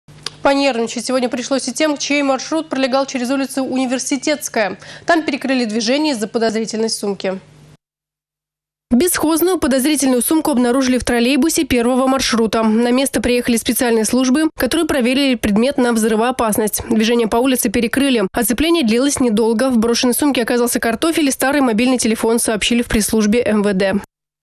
Обнаружили бесхозную подозрительную сумку в троллейбусе
Понервничать сегодня пришлось и тем, чей маршрут пролегал через улицу Университетская. Там перекрыли движение из-за подозрительной сумки.